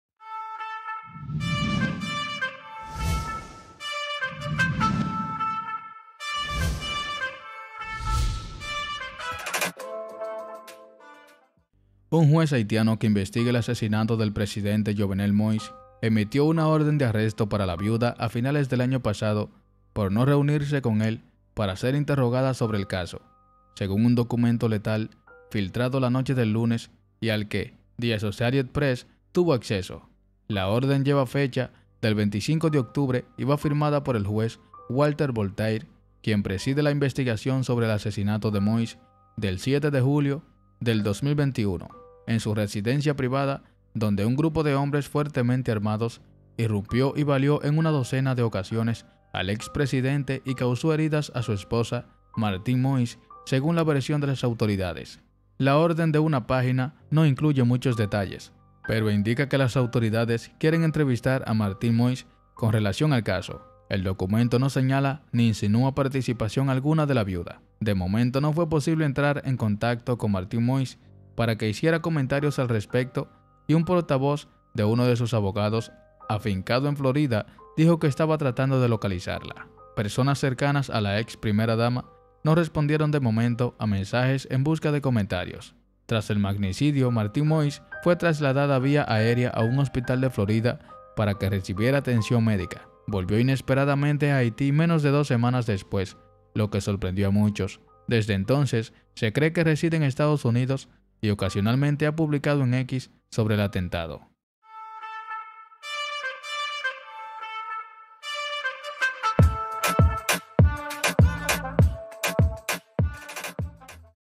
Caso Jovenel Moïse: juez emite ORDEN de ARRESTO contra VIUDA de presidente haitiano ASESINADO
La orden fue emitida contra Martine Moïse por no acudir a reunirse con el juez.